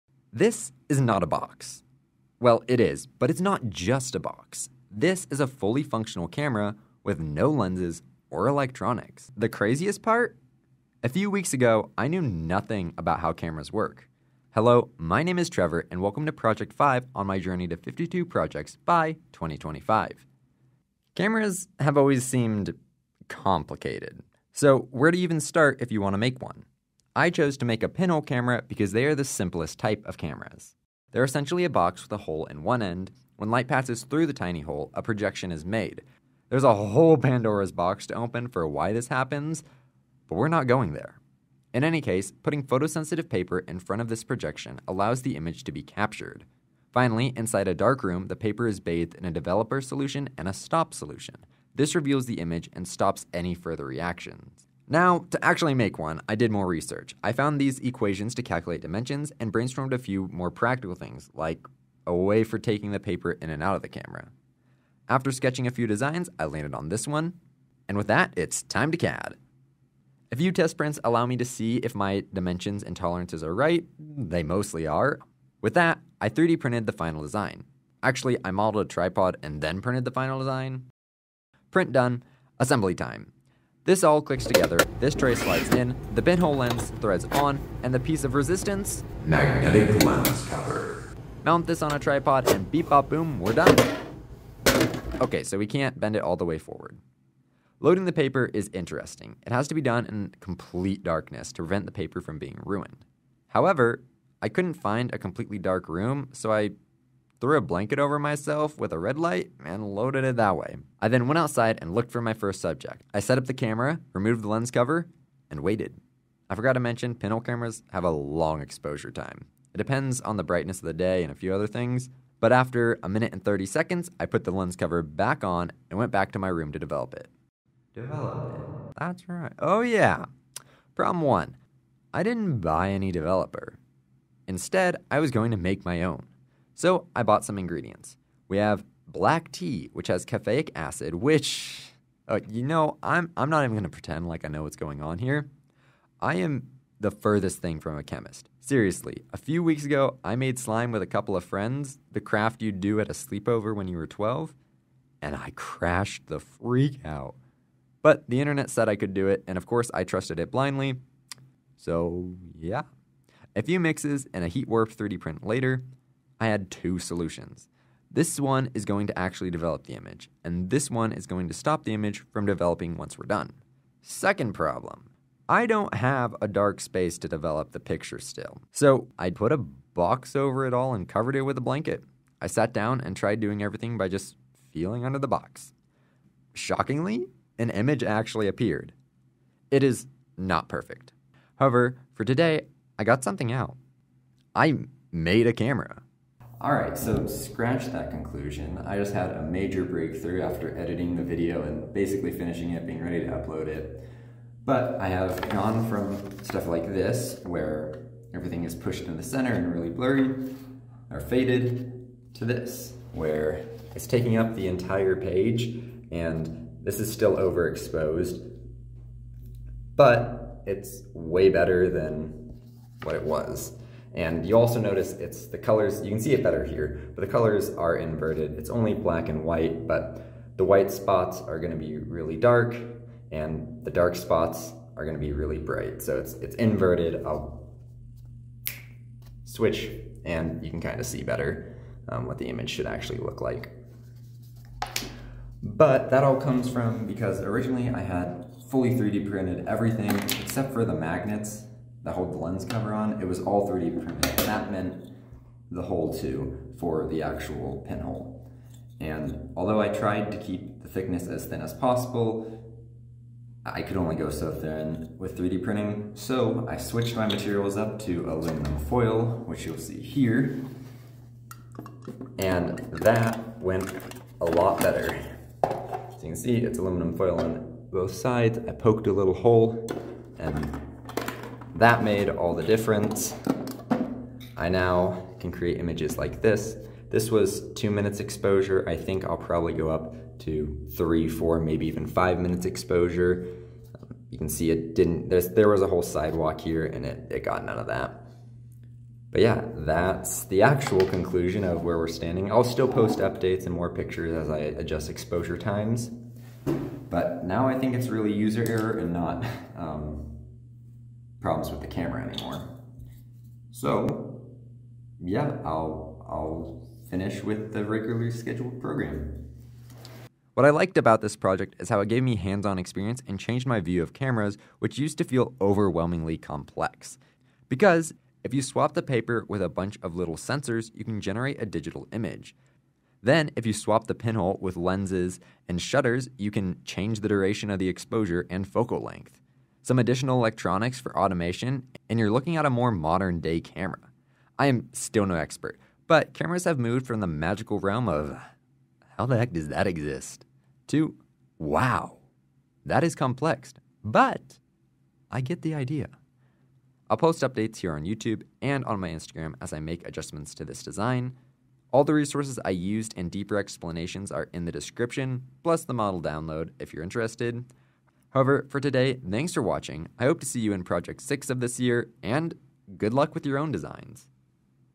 How I 3D Printed a Pinhole Camera
Want to build your own pinhole camera? ↓↓↓

**INTRODUCTION**
Recently, I have been learning about cameras, and decided to take on the challenge of making one of my own. This is my most advance project so far! It took a little over a week to make. (My goal is to generally keep these designs to a week long, so I can hit 52 projects by 2025).

I'd really appreciate it if you downloaded this model and gave it try!

Photo Paper
- This photographic paper has worked really well! It is a reasonable size, and held up great.
- Used for the magnetic lens cover (and much more); I have used these magnets in every project since I bought them.
Hatchbox PLA
- I love Hatchbox filament. Given the choice, it is solely what I use. (However, for this project, I ended up using another random filament that was black).
Aluminum Foil
- Do NOT try printing the pinhole... It did not work welll. Aluminum foil worked great however!
Prusa MK4 3D Printer
- I can't imagine, you're gonna go out of your way to buy a 3D printer for a pinhole camera (don't). You can make a pinhole camera out of almost anything. However, if you are looking for a 3D printer anyways, I recommend a Prusa Printer.

*A note on the developer and stop solution: Missing from the list above is these chemicals. You will need them. I made my own, and the video I used is in the next section; however, I would recommend just buying the solutions (make sure to handle them with care). The quality of the DIY developer and stop shows... My next improvement will likely be getting an actual developer and stop.

**WANT TO MAKE YOUR OWN**
I fully support taking up this project on your own! I have learned a lot about cameras and optics through designing this pinhole camera. Below are the resources I used to design my own, and a little about the science behind pinhole cameras:

- This video is from a pinhole enthusiast! It gives a great introduction to pinhole photography, and some of the reasons people enjoy it.

- This is the video I used to make my developer and stop. Going back I would just buy a developer and stop solution. The solutions here worked for me, but is tedious to make and the images have begun to fade over time.

- I have not used the method in this video yet, but plan to in the future to make photo negatives.

- This video covers some of the mathematics behind pinhole photography. Unfortunately, I found that manufacturing pinhole diameters to an accurate size is difficult, and ultimately the actual dimensions just had to be close-ish to math.

**THE SCIENCE BEHIND PINHOLES**

Okay, time to open that "pandora's box" - Why does the pinhole project an image? From my understanding - please correct me if I am wrong - as you approach a smaller and smaller pinhole, only the light travelling in a straight line can get inside the box. This stops light from different angles intersecting each other and simply illuminating the inside of box. Instead, distinct points of light enter the box, allowing an image to be formed.

What is the FOV? The field of view is determined by the focal length - the distance between the pinhole and the paper. At some "normal" length, the image's FOV will match what you see. Squishing in past this distance will result in a wide FOV. Alternatively, stretching out will zoom the FOV in. The equation for calculating the "normal" focal length that I found was sqrt(paper length * paper height). If you have additional math question please reach out - in the comment or any other method. I would love to help and/or direct you to the resources I used!

Why is the image inverted? It has to do with chemistry - which I suck at - but essentially the paper is coated with light reactant chemicals. Placing this in front of the projection created by the pinhole allows these chemicals to react. During this phase the darker parts of the projection react less, and the lighter parts react more. This creates an "invisible" image. To make it visible you bathe the paper in a developer. This solution reacts with the remaining chemical creating dark-spots. However, the parts with the most remaining chemicals are the spots that got the least amount of light, so the image is inverted. There's a clever work around in one of the above videos to create a photo negative. (You can also take a picture with your phone and invert it).